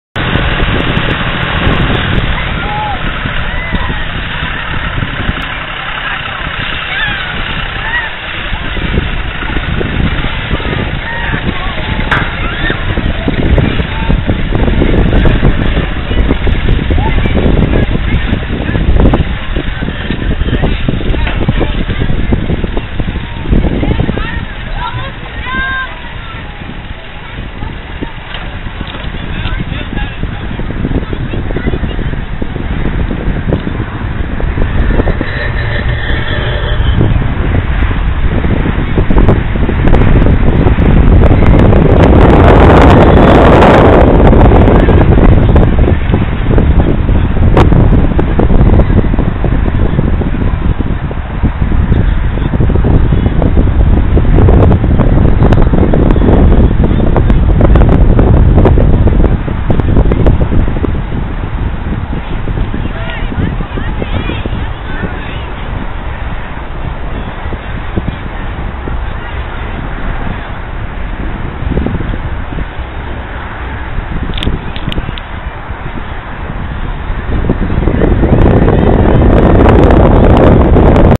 Farm Haven Hayride
Blimp cam video of the hayride coming back to the barn.